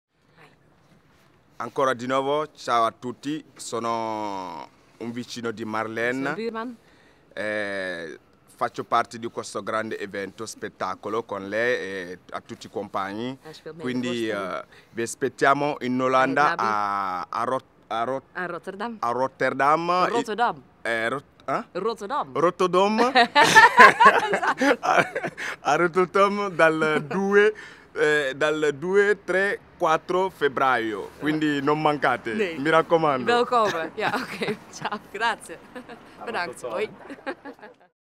Wunderbaum - Wie is de echte Italiaan? Nummer 4
WIE IS DE ECHTE ITALIAAN?
CHI È IL VERO ITALIANO?
NUMMER 4
Buurman Naby Camara.
Woont al 12 jaar in Italië.
Getrouwd met een Italiaanse, heeft 2 kinderen en speelt balafon!
Balafon?
Ja.
Balafon.

2, 3 en 4 februari 2017 in Zaal Ro Theater